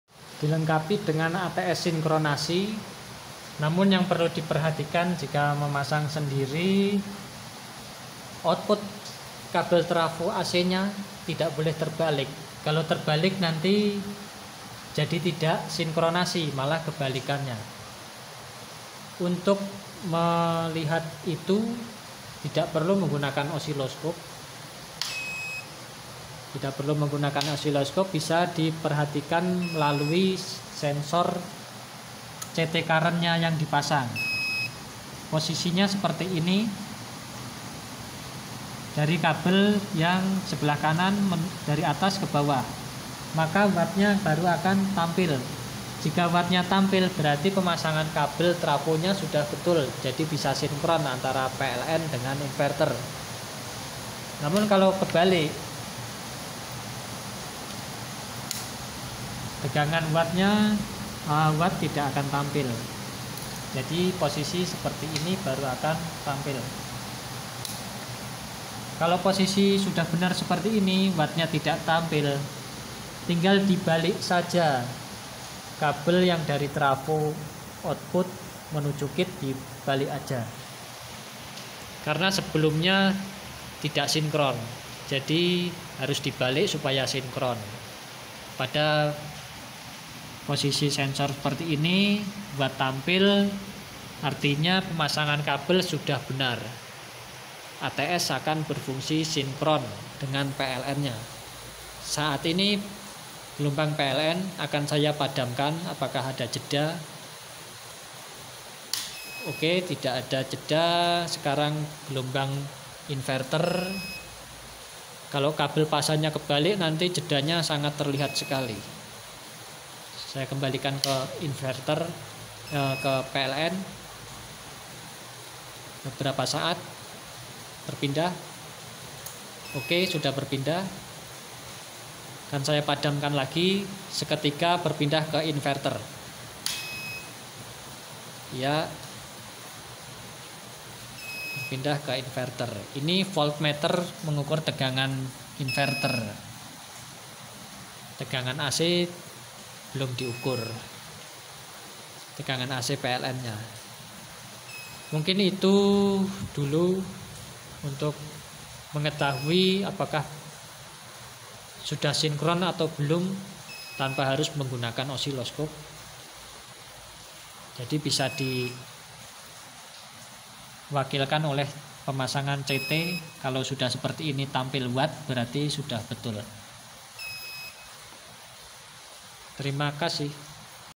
Posisi pemasangan CT Current sensor agar ATS inverter sinkronasi dengan PLN
terlihat kedip saat perpindahan karena PLN sempat padam dan ada perbedaan tegangan PLN dgn inverter. itu umumnya UPS standby, namun sudah sangat cepat dibawah 10ms

untuk sistem perpindahan inverter - PLN / PLN - inverter , ketika dua sumber ada, tdk ada kedip sama sekali.
karena kedua sumber tdk sempat padam.

inverter ini jg bs bekerja sistem UPS online dimana untuk digunakan pd beban peralatan server yg sangat sensitif. sistem kerja UPS online dimana inverter selalu menyuplay beban AC dan suplay DC inverternya di support terus menerus oleh PLN yg mengisi baterai/menyuplay inverter